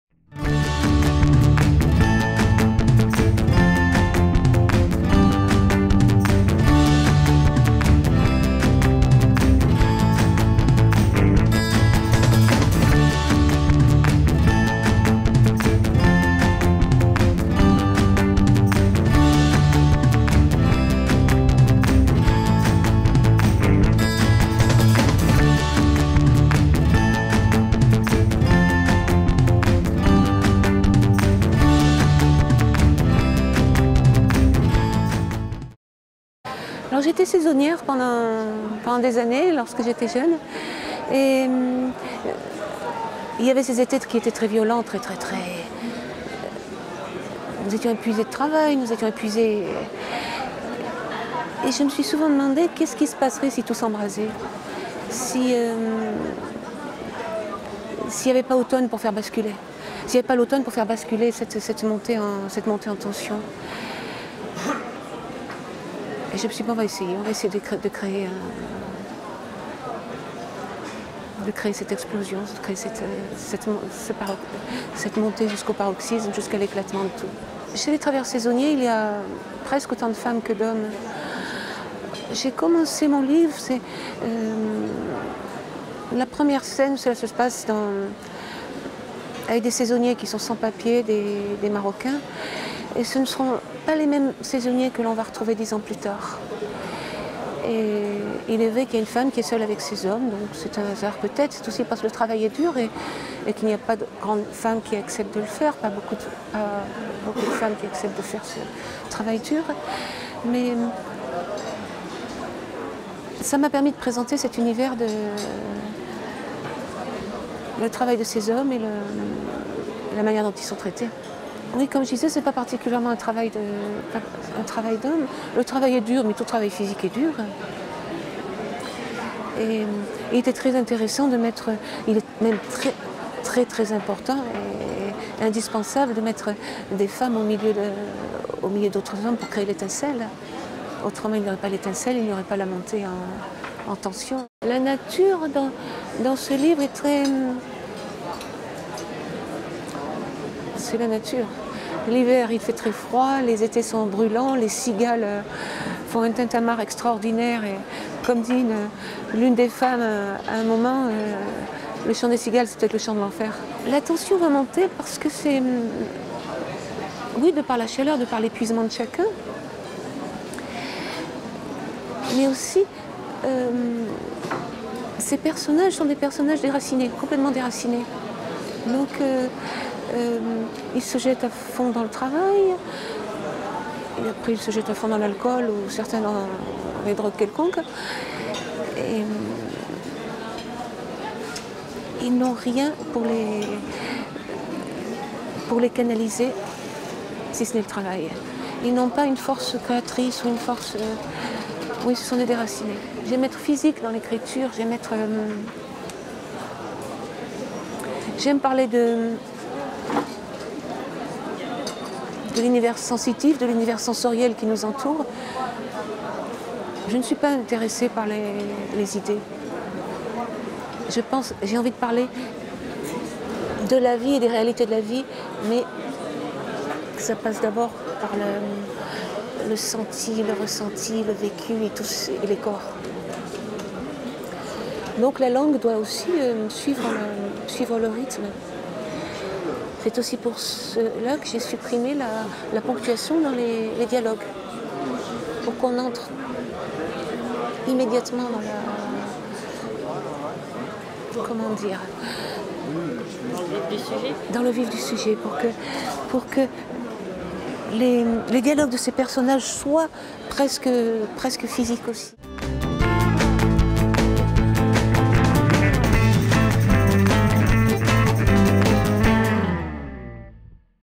Catherine Poulain - Le coeur blanc
À l'occasion des Correspondances de Manosque, rencontre avec Catherine Poulain autour de son ouvrage "Le coeur blanc" aux éditions de l'Olivier. Rentrée littéraire automne 2018.